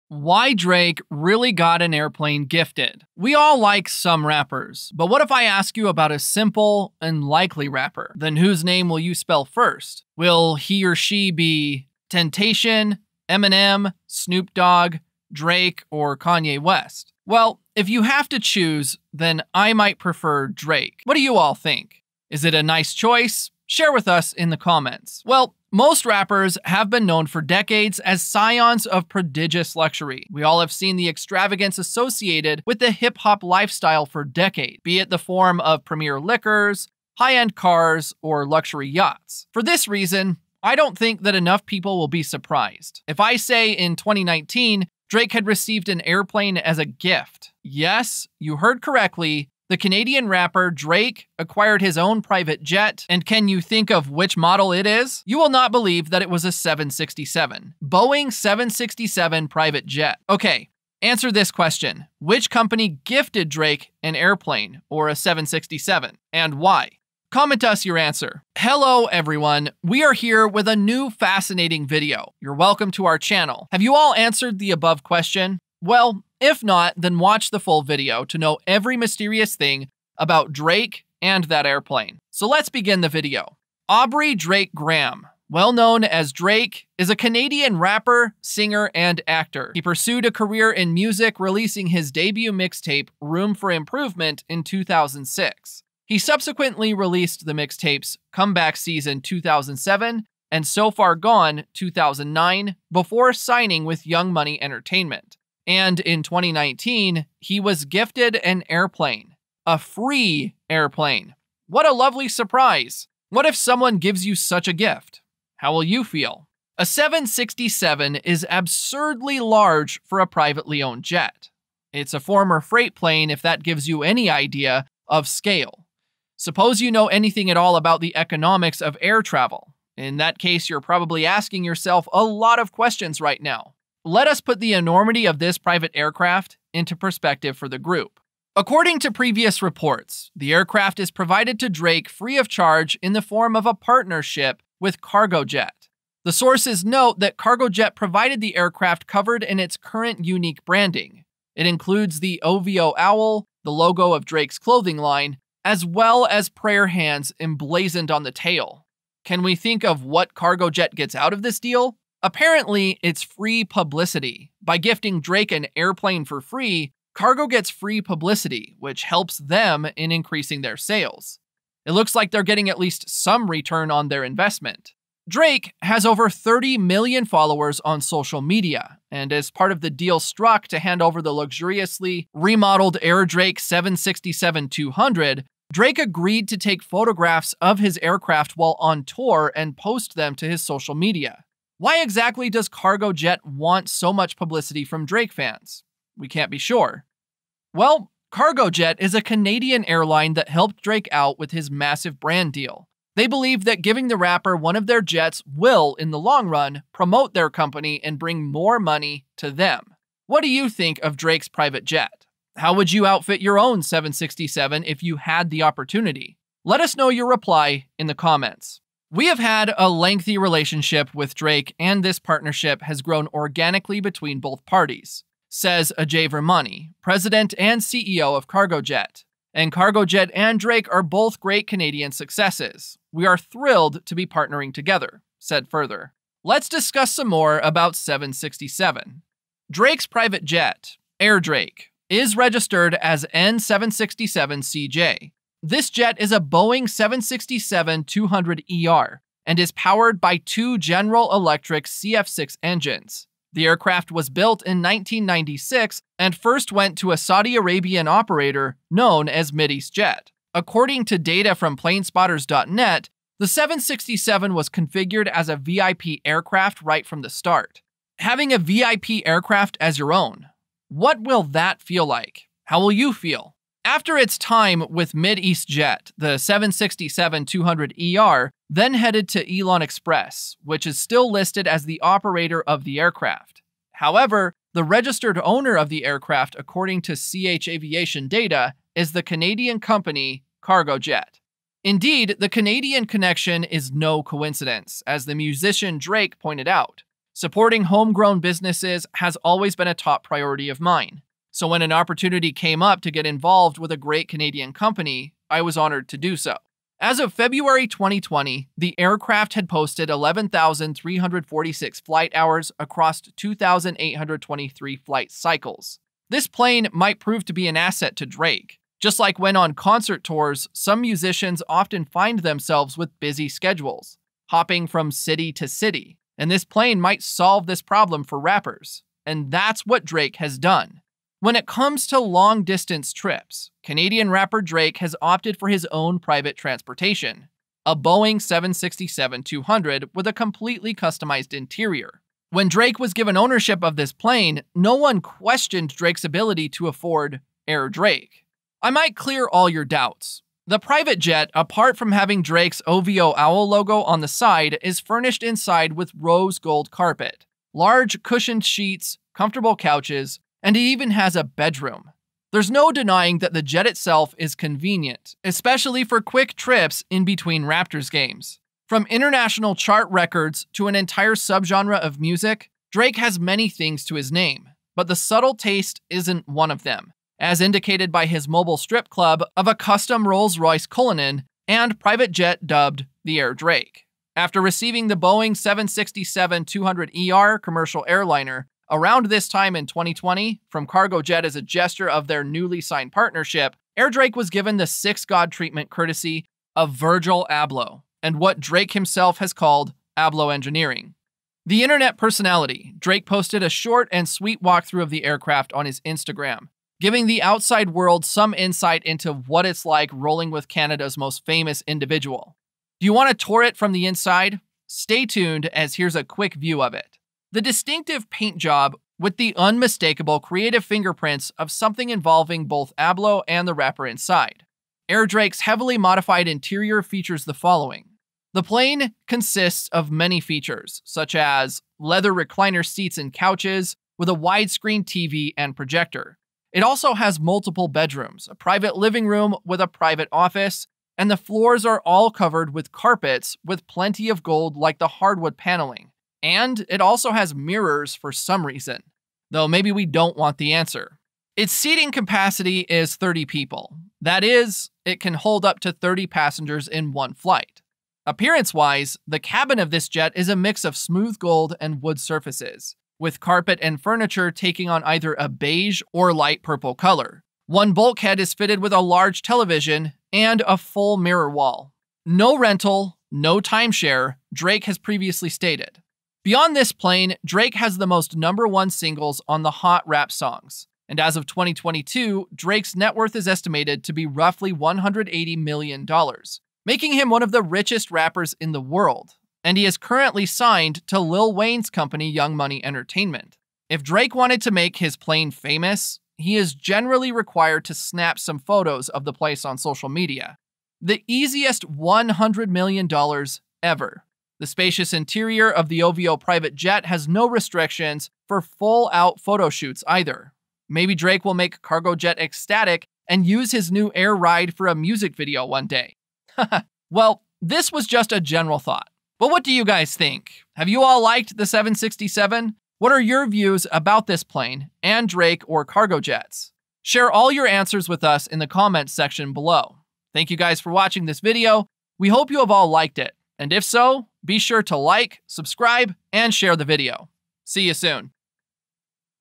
Why Drake REALLY GOT An Airplane Gifted!
We all like some rappers, but what if I ask you about a simple and likely rapper, then whose name will you spell first? Will he/she be, tentacion, Eminem, Snoop Dogg, Drake, or Kayne West. Well, if I have to choose then I might prefer Drake. What do you all think, is it a nice choice? Share with us in the comments.

Well, most rappers have been known for decades as scions of prodigious luxury. We all have seen the extravagance associated with the hip-hop lifestyle for decades, be it in the form of premier liquors, high-end cars, or luxury yachts.

For this reason, I don't think that enough people will be surprised. If I say in 2019 Drake had received an airplane as a gift!! Yes, you heard correctly, the Canadian Rapper Drake acquired his own private jet, and can you think of which model is it?

You will not believe that it was a 767, Boeing 767 private jet. Okay, answer this question, which company gifted Drake an airplane or a 767? And why?? Comment us your answer!!

Hello everyone, we are here with a new fascinating video. Have you all answered the above question?